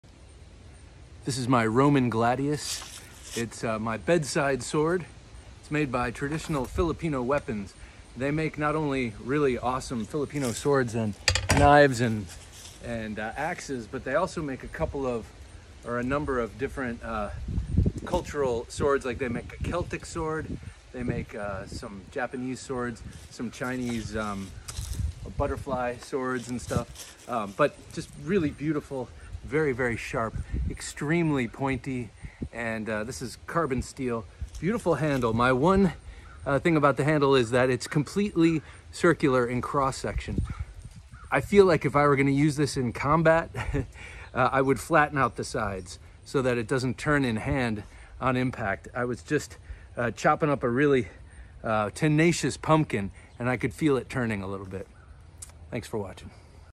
Bedside Sword - Knife Junkie
Roman Gladius by Traditional Filipino Weapons -- The Knife Junkie's bedside sword.

🔪 SHOPPING FOR A KNIFE 🔪
The Knife Junkie Knives for Sale Webpage --